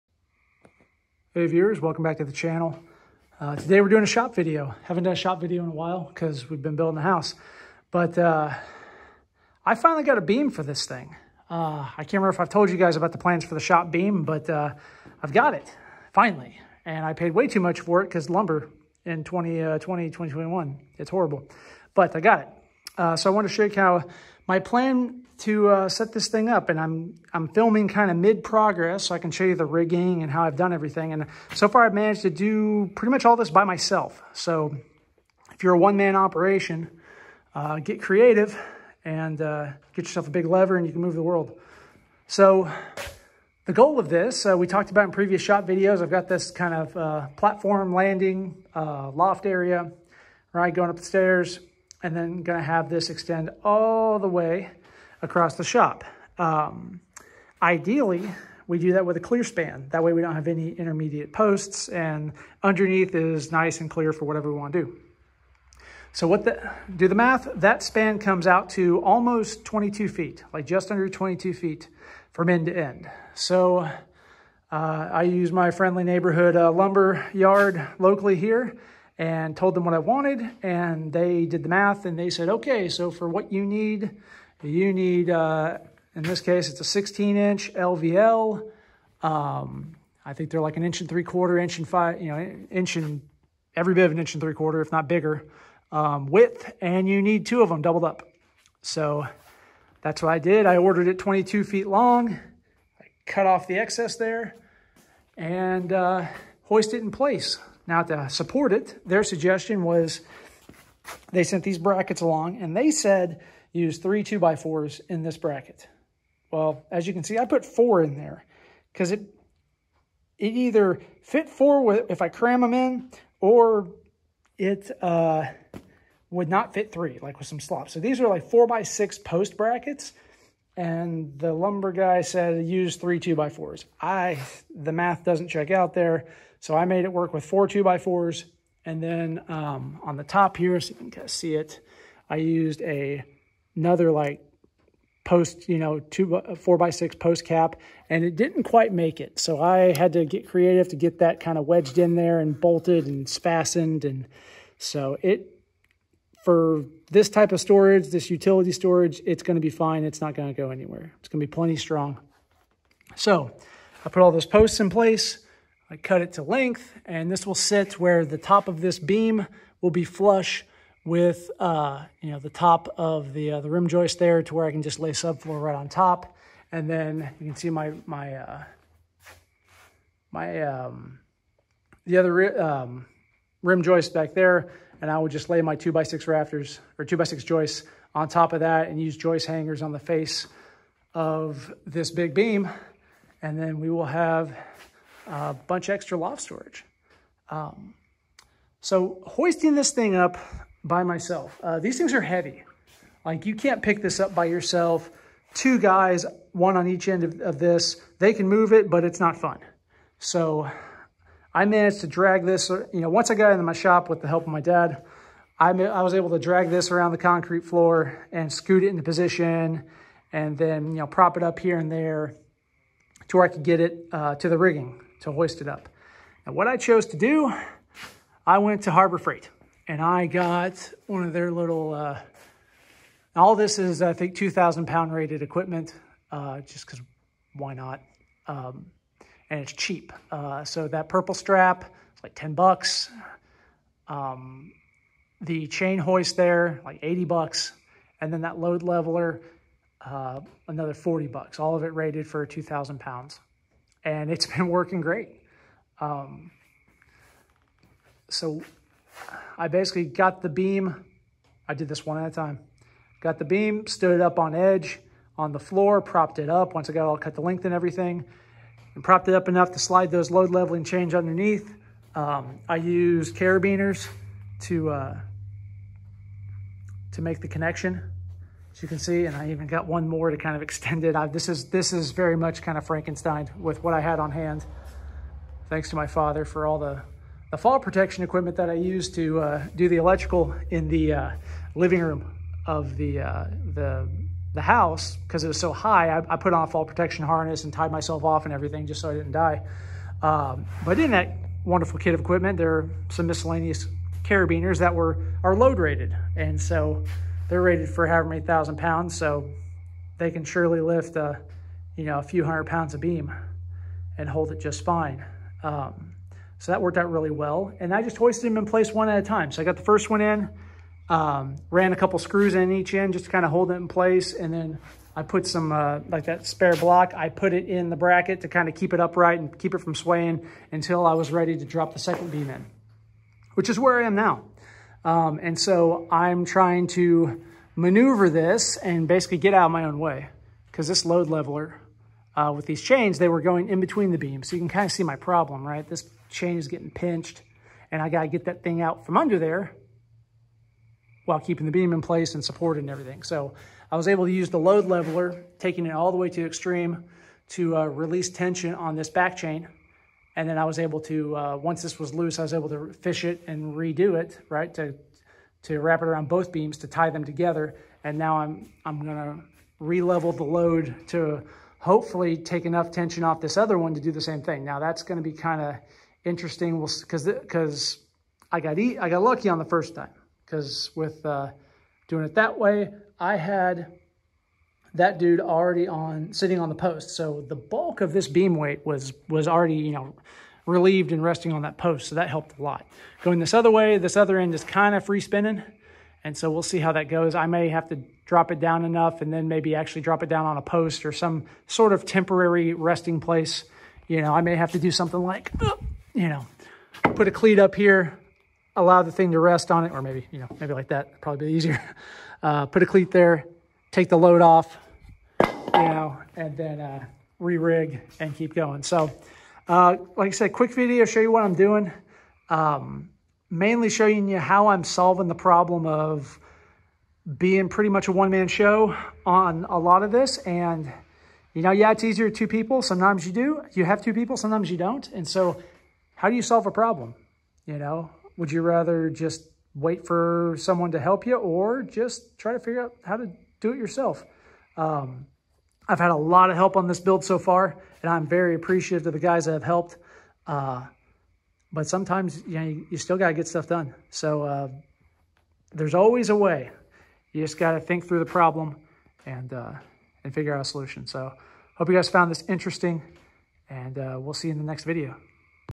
Episode 57 - Hoisting a LVL beam by yourself
In this episode I switch back to the shop and discuss hoisting and placing a large beam by yourself.  This can be a 1-man job with the right tools and planning.

If you want to watch this channel on a YouTube alternative, please check us out on Odysee.